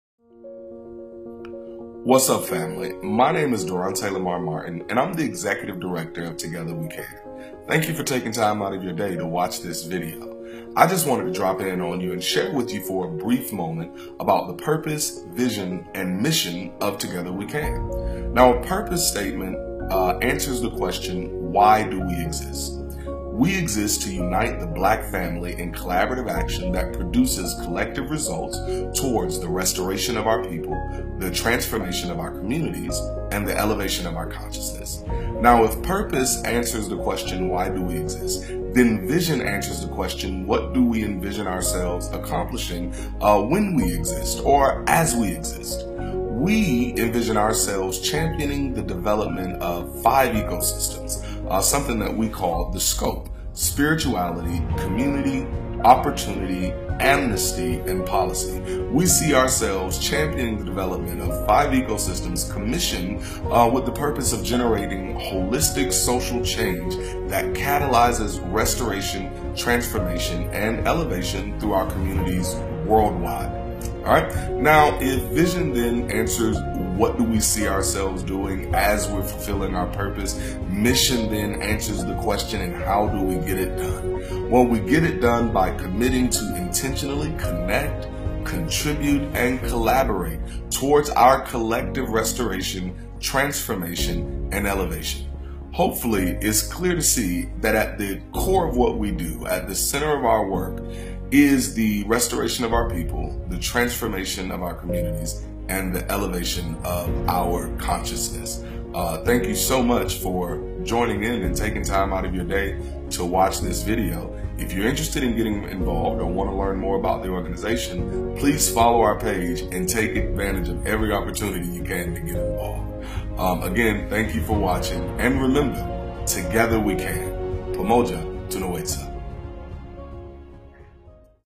Purpose Mission and Vision| TWC
Darante' LaMar shares the Vision of Together We Can